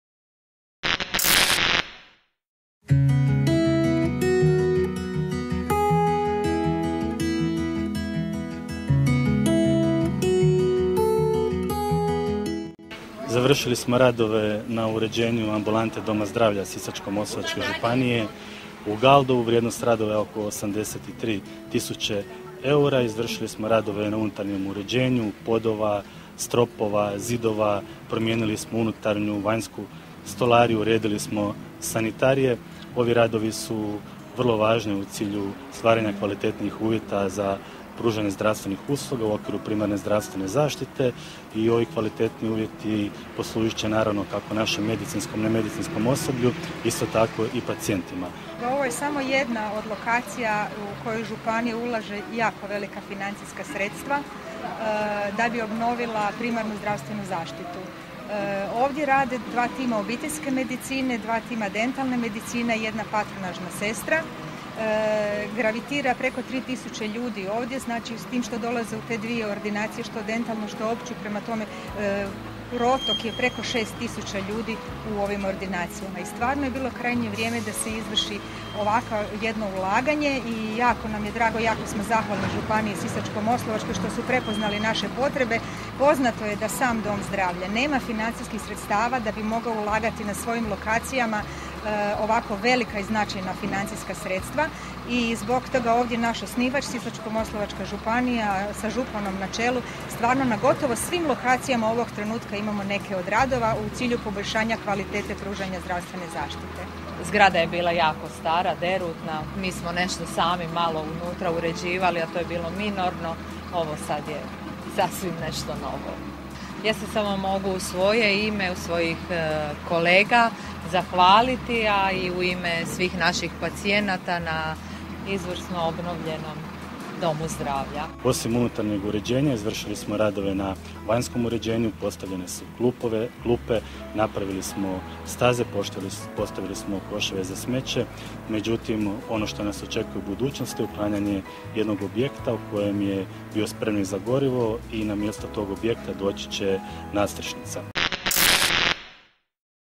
OBNOVLJENA AMBULANTA U GALDOVU
Ambulanta u Galdovu je samo jedna od lokacija primarne zdravstvene zaštite Doma zdravlja Sisačko – moslavačke županije u čiju je obnovu Županija uložila oko 83 tisuće eura. Ovoj ambulanti gravitira oko 6000 pacijenata svojim liječnicima opće i dentalne medicine koji će sada imati bolje uvjete.